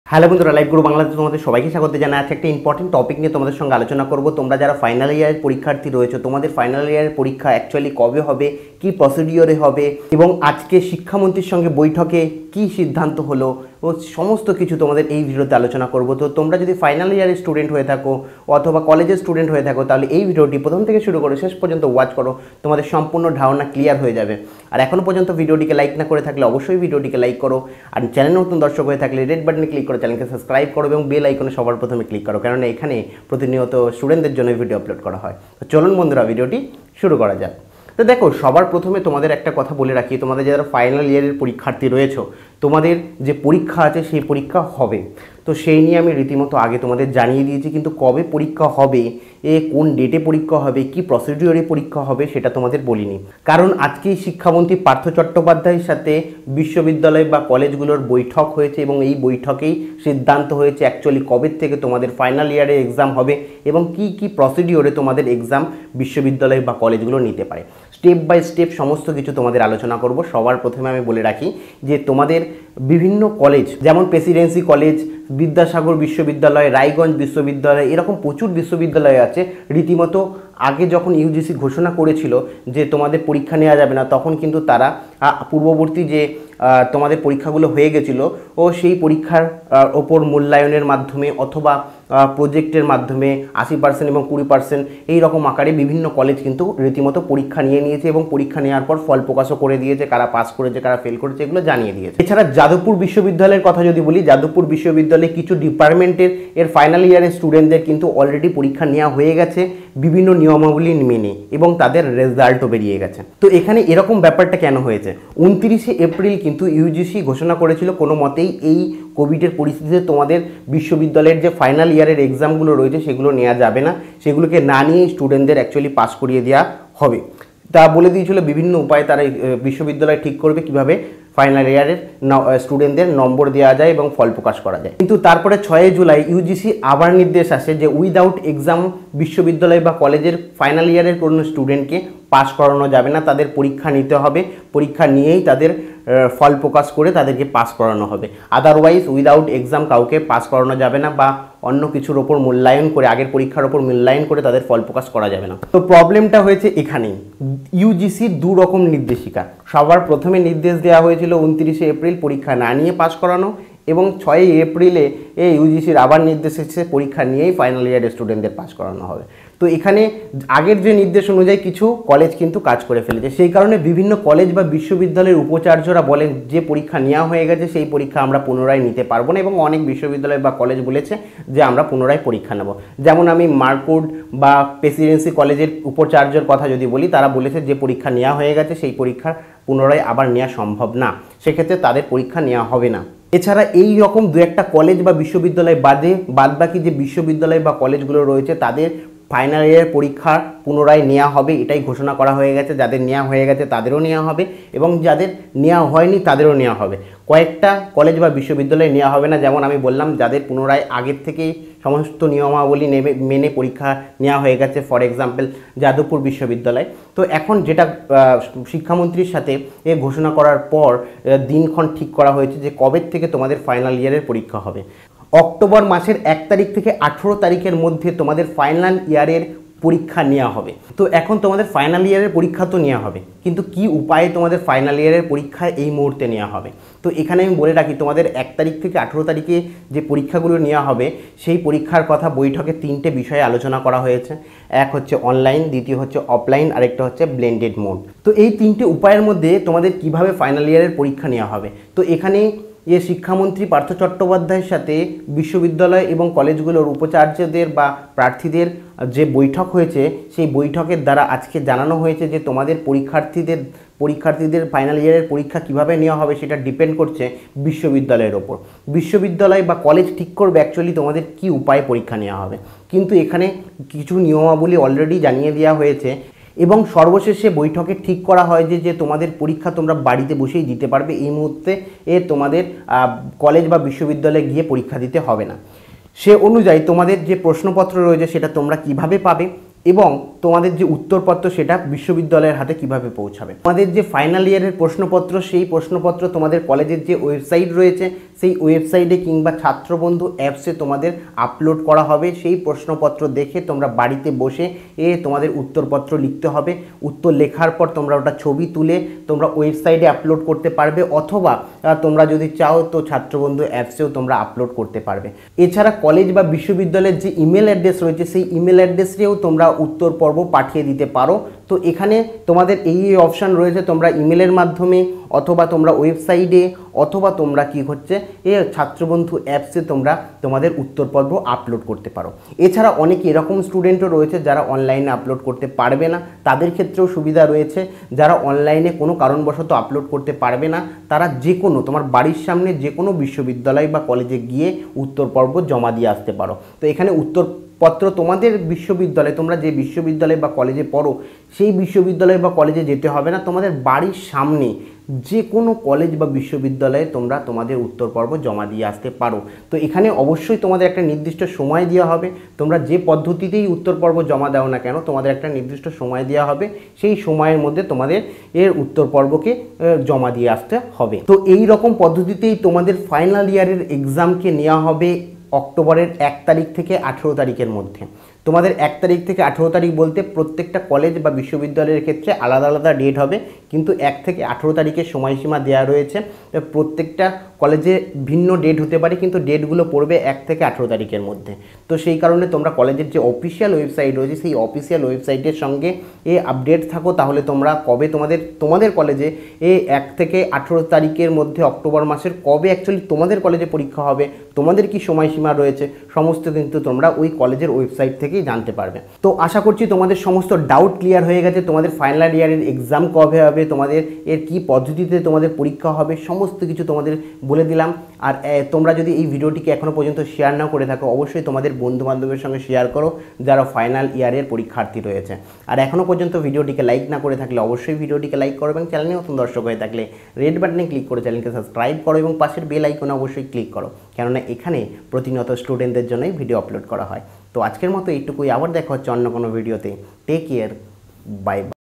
ফাইনাল বর্ষের পরীক্ষা কবে কিভাবে হবে | west bengal final year exam news today
west bengal final year exam news today

🔴 নমস্কার বন্ধুরা , তোমাদের  Life Guru Bangla তে স্বাগত।আপনারা যদি বাংলা ভাষা কে ভালো বসেন আর শিক্ষামূলক দেখতে চান ।